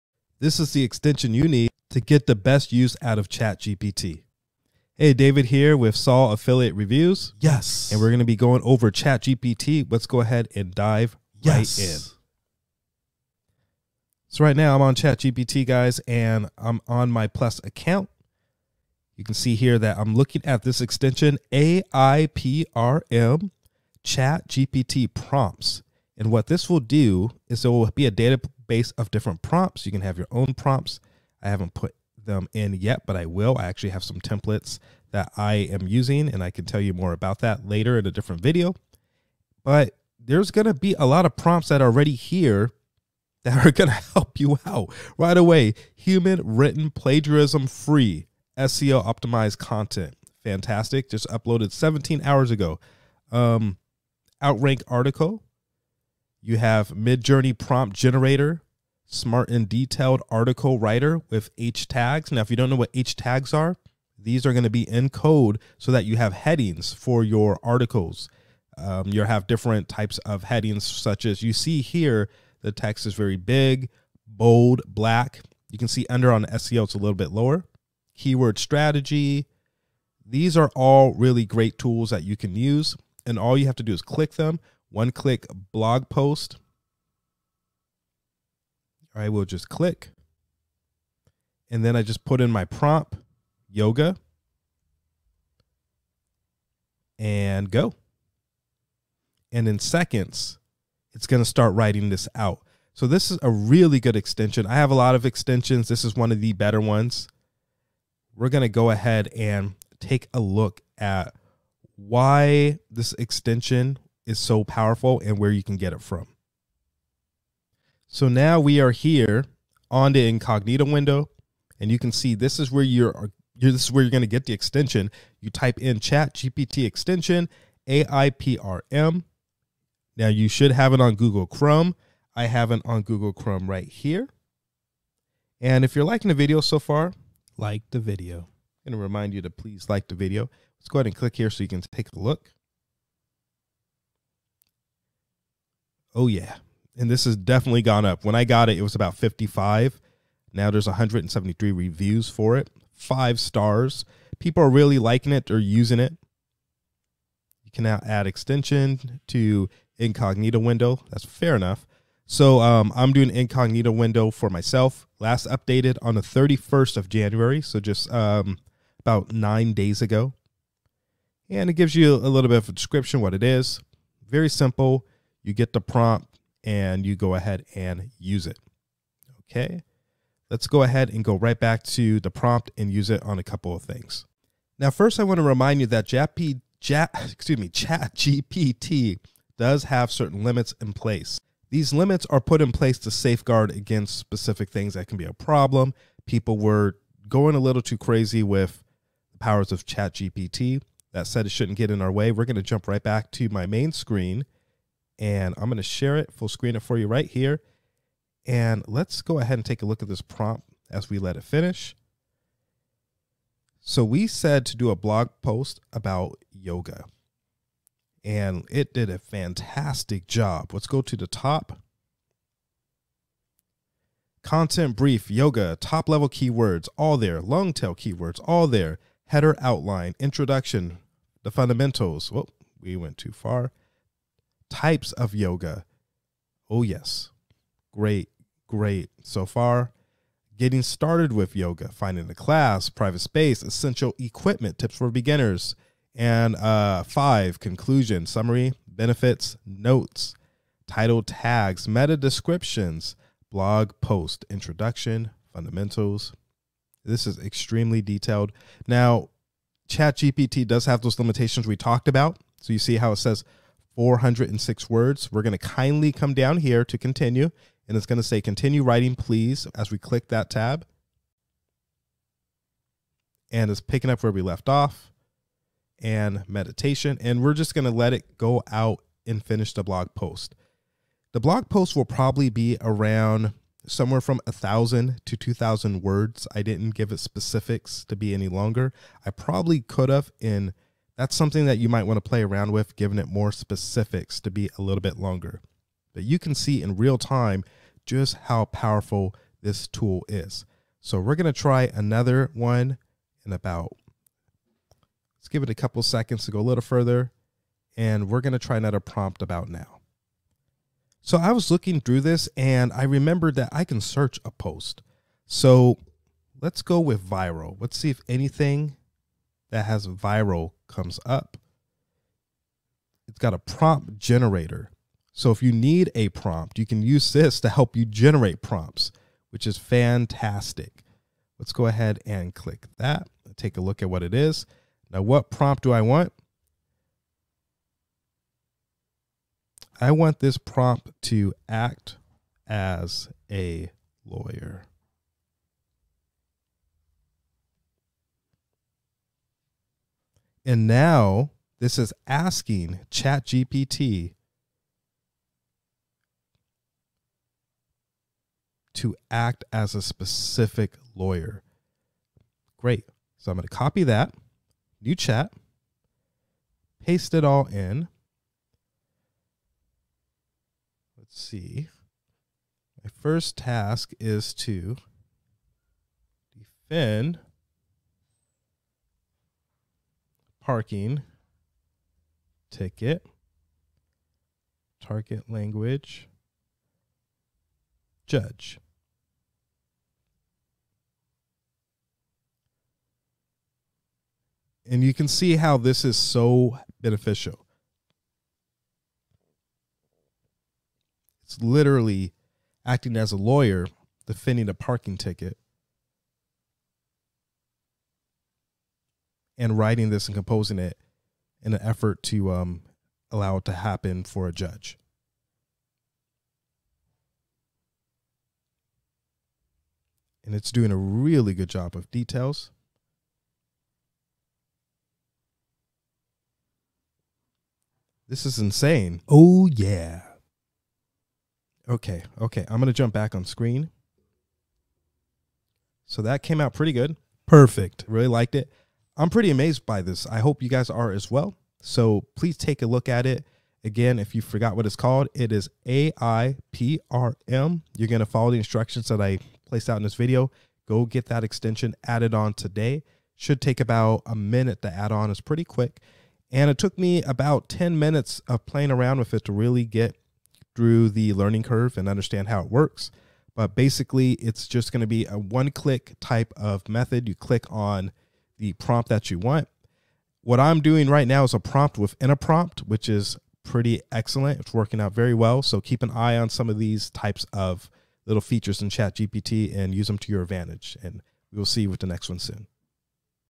Unveiling the New AI-Powered GPT Chat AIPRM Chrome Extension - Here's What You Need to Know!
Have you been looking for a way to easily talk to your customers on your website or via chat? If so, you'll love our new AI-powered GPT chat extension! In this video, we'll show you all the details about this exciting new product, and how you can start using it today.

Not only is our chat extension easy to use, but it's also powered by AI. This means that it can intelligently respond to your customers' queries, and handle difficult conversations with ease. So if you're looking for a way to improve your customer service, or simply want to add a layer of automation to your chat process, then you're in luck!

We hope you enjoy this video, and please